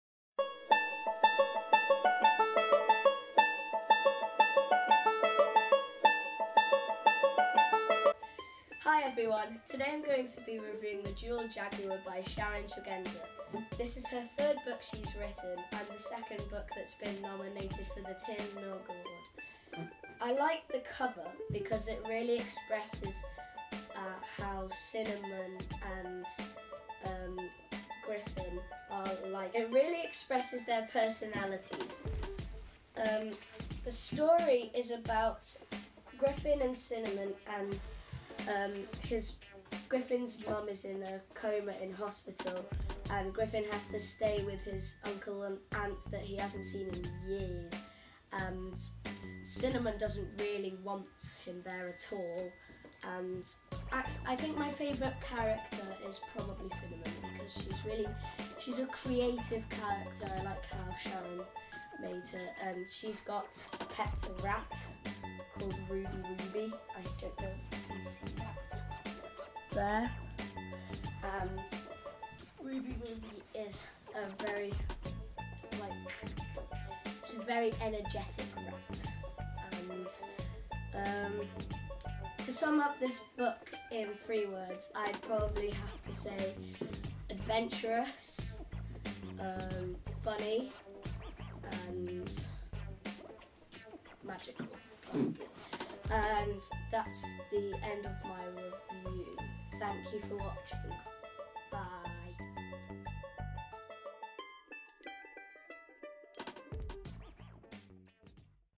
The Jewelled Jaguar Vlog Review
The Jewelled Jaguar by Sharon Tregenza was nominated for the Tir-na-nOg Award in 2018. Here, Noah takes a look at the book and gives his verdict.